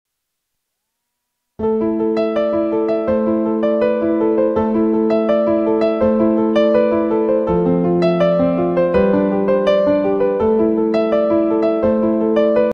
lego short bluecoat vs redcoat
a battle between a mean blue coat and 2 red coats